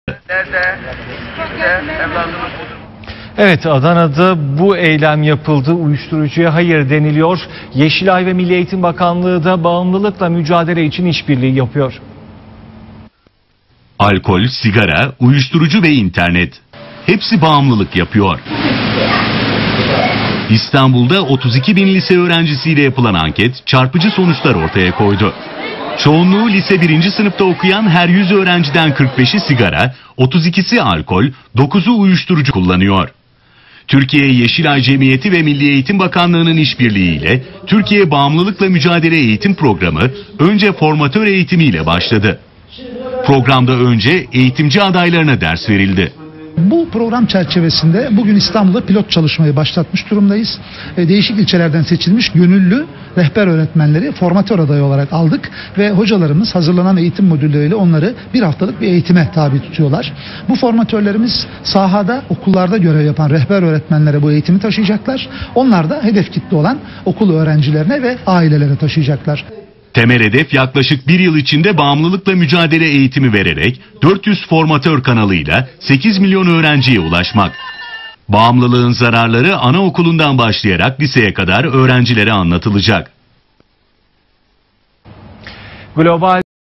Yeşilay Genel Başkanı M. İhsan Karaman TRT Haber'e TBM projesini değerlendirdi
Türkiye Bağımlılıkla Mücadele Eğitim Programı